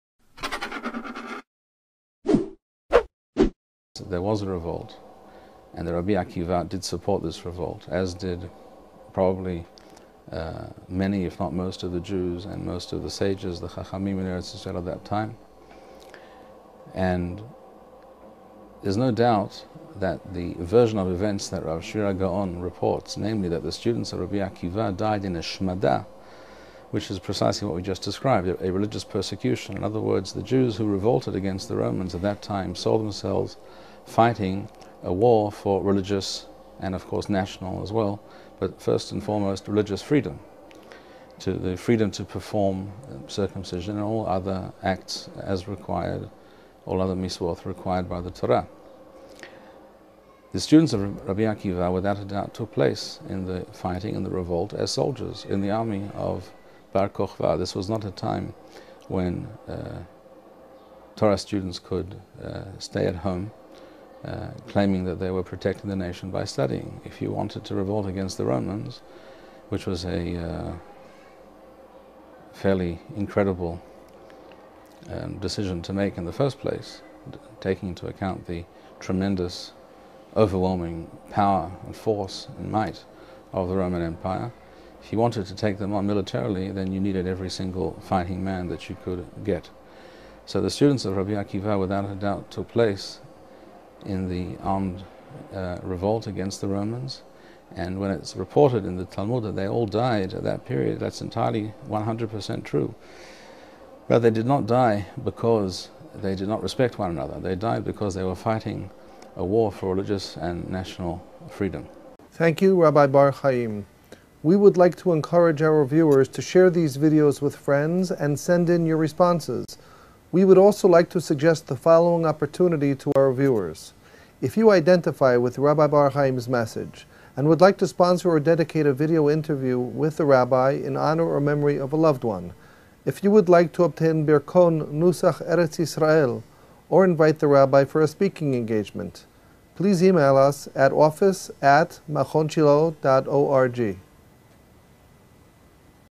Sefirat HaOmer: The Revolt of the Students of Rabbi Akiva
Interview with the head of Machon Shilo, Rabbi David Bar-Hayim